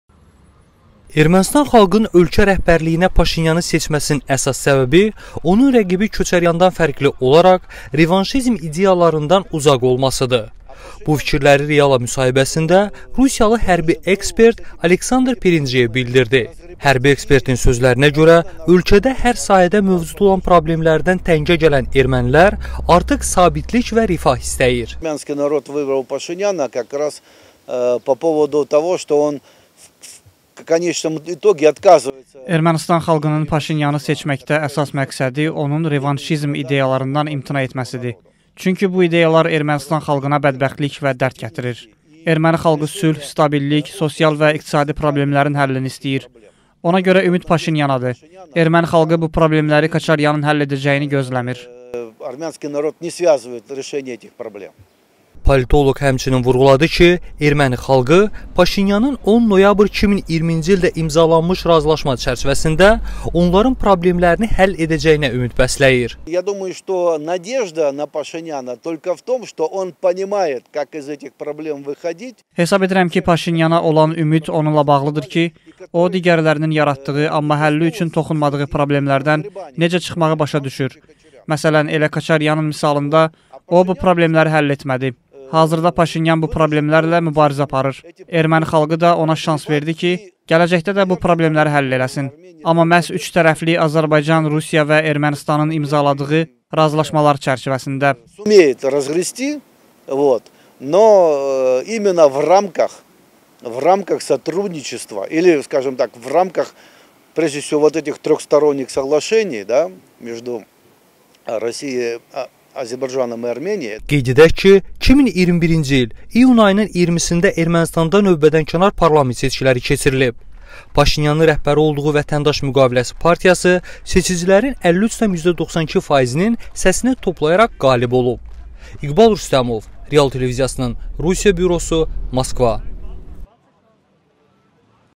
Ermənilərin seçiminə Rusiyadan rəy
A.Perendjyev: “Erməni xalqı revanşizm yox, problemlərin həllini istəyir”